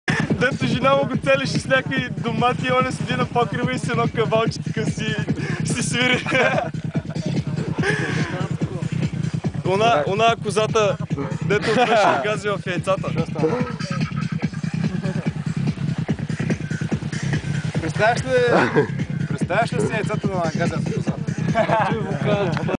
SUN Fields II - Miro talks about Purko from "Mr. for a day" movie
The second SUN Fields party was near Sveti Vlas - between Suny Beach and Nessebar @ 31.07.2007. It was completely unprepared and in the same time absolutely perfect experience.